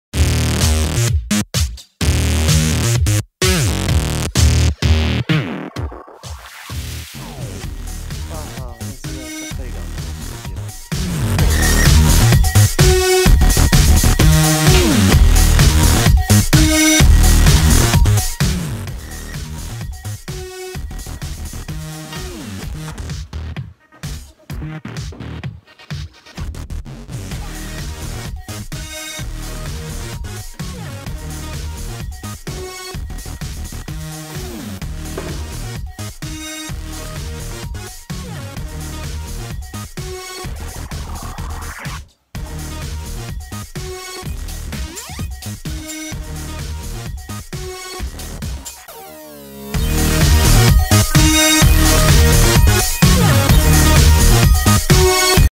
Gucci marmont belt
Hi guys!! This is one of my Christmas gifts 💕 unboxing for you ⚡️ like me please 👍